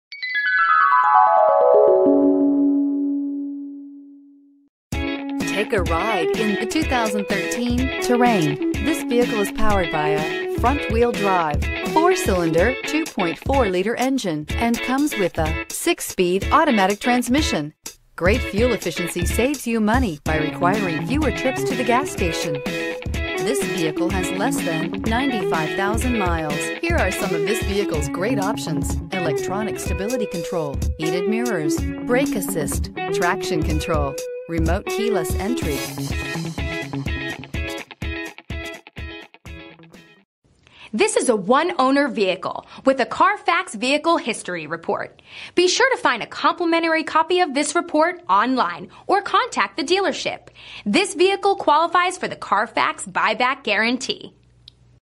2013 GMC Terrain 864419A
2013 GMC Terrain SLT-2

2013 GMC Terrain SLT-2 FWD 2.4L 4-Cylinder SIDI DOHC VVT Leather, 32 Miles per Gallon, 18 x 7.5 Chrome-Clad Aluminum Wheels, Power moonroof, Power Programmable Rear Liftgate, SIRIUSXM Satellite Radio.   Awards:   * 2013 IIHS Top Safety Pick   Reviews:   * If youre looking for something akin to a Ford Escape or Honda CR-V, but you dont like futuristic styling or 4-cylinder powertrains, the GMC Terrain for 2013 may very well be the answer. Its squared-off edges and flared wheel openings say GMC Truck loud and clear. Source: KBB.com   * Premium look inside and out; quiet cabin with lots of storage bins; spacious and adjustable backseat; comfortable highway ride. Source: Edmunds